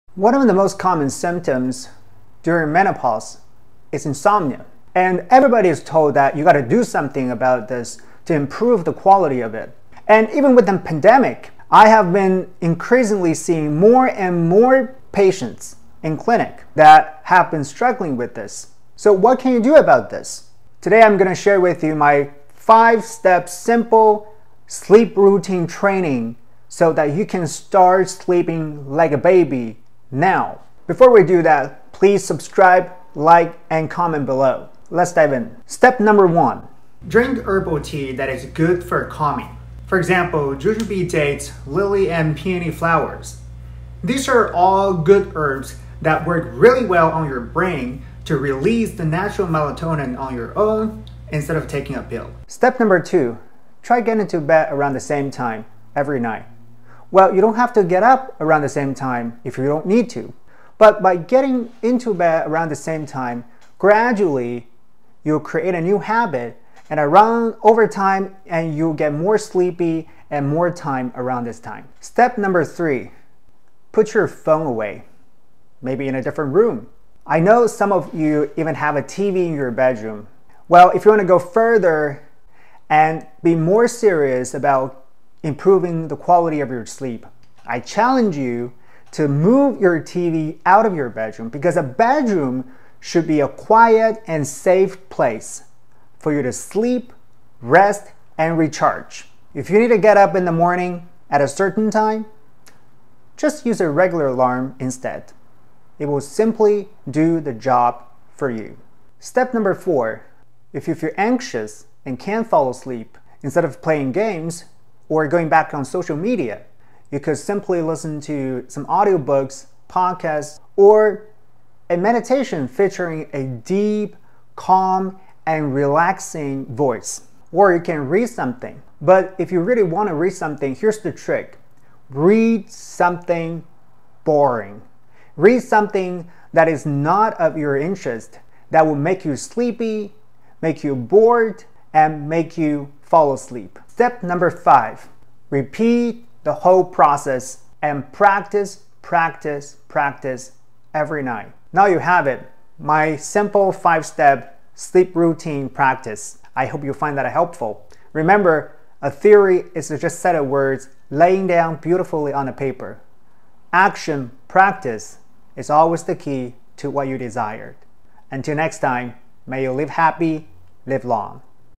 Menopause and Sleep. How to sleep like a baby without medication
Going through menopause and can’t sleep? You’re not alone. On average, around 12 percent of women experience sleep complaints. Not only do women have hard time falling asleep, but staying  asleep, and what's worse a lot of time is sleep issue often comes with night sweats, frequent urination, etc. Not getting enough sleep can affect all areas of life. In this video, I give you my simple 5-step sleep routine exercise to help you sleep through the night without medication.

🔥 Watch my FREE TRAINING to learn how to ease menopausal symptoms and start living Symptom-Free